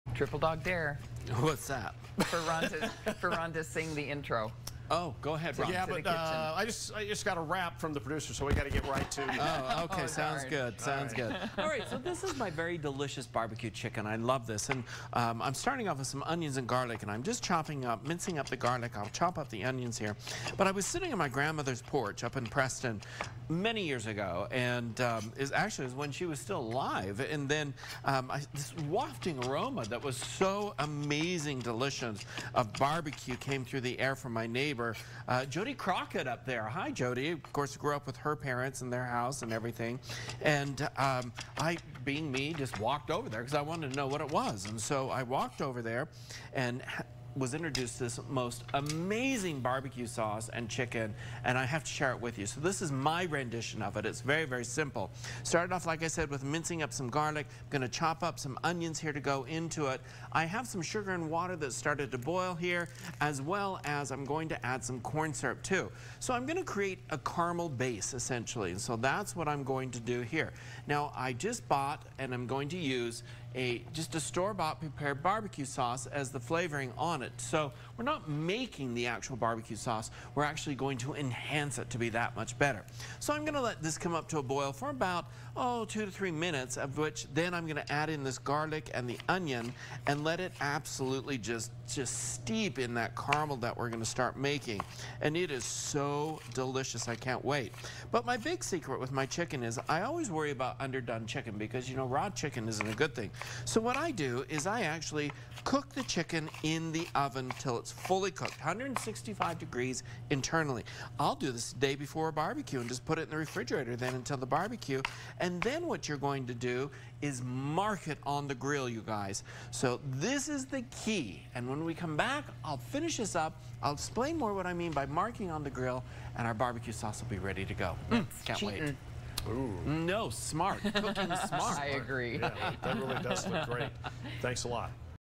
Cooking with Chef Bryan - BBQ Chicken with Incredible BBQ Sauce
BBQ chicken is delicious, but it’s this BBQ sauce that makes it amazing. Simple to make, this sauce adds the right sweet and spicy kick to your favorite summertime bbq chicken!